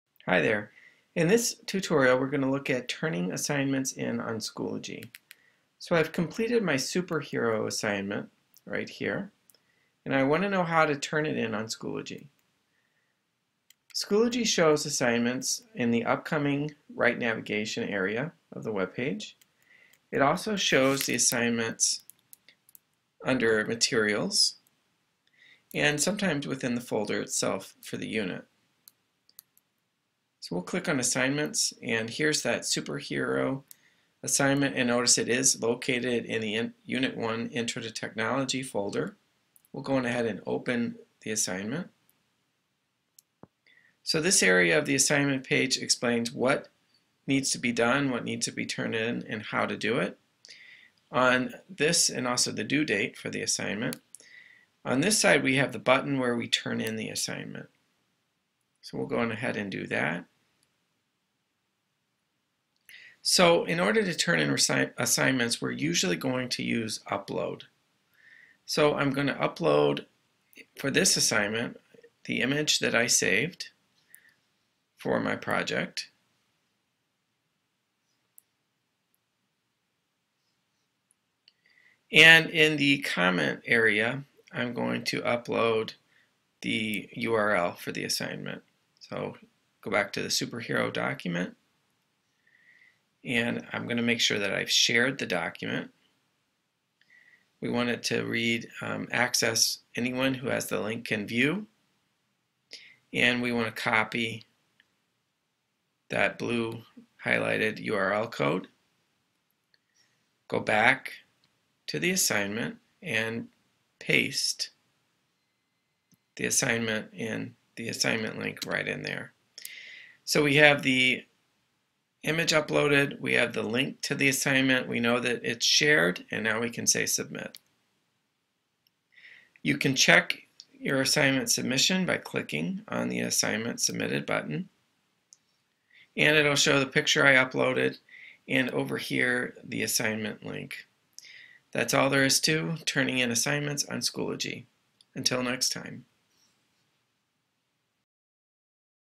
Turning in Assignments on Schoology
Tutorial shows how to turn assignments in on Schoology